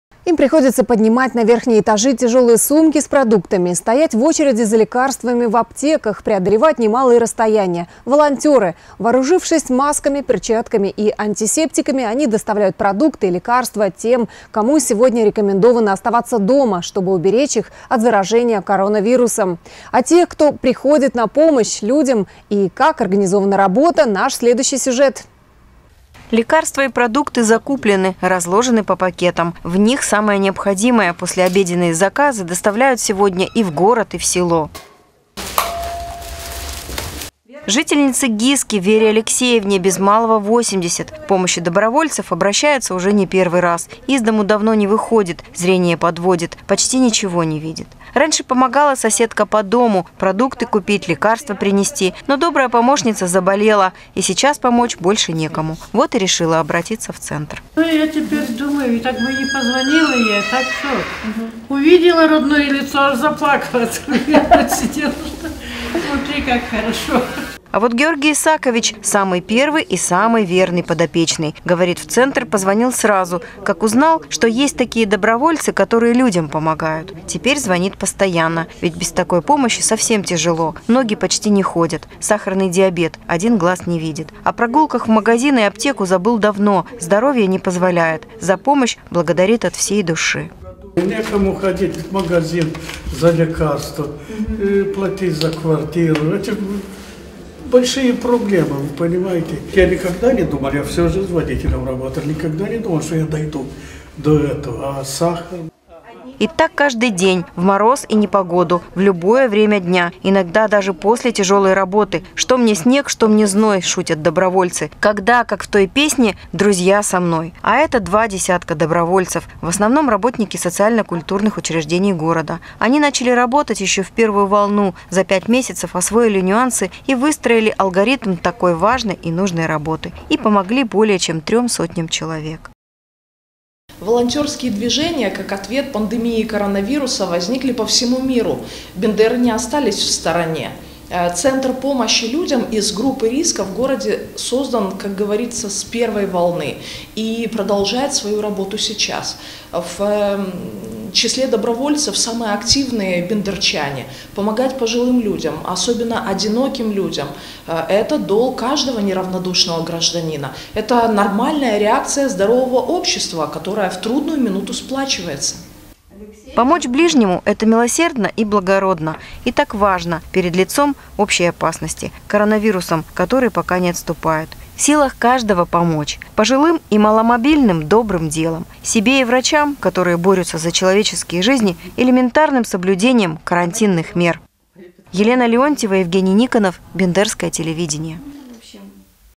Доброе дело
Им приходится поднимать на верхние этажи тяжелые сумки с продуктами, стоять в очереди за лекарствами в аптеках, преодолевать немалые расстояния. Волонтеры, вооружившись масками, перчатками и антисептиками, доставляют продукты и лекарства тем, кому сегодня рекомендовано оставаться дома, чтобы уберечься от заражения коронавирусом. О тех, кто приходит на помощь людям и как организована работа, – наш следующий сюжет.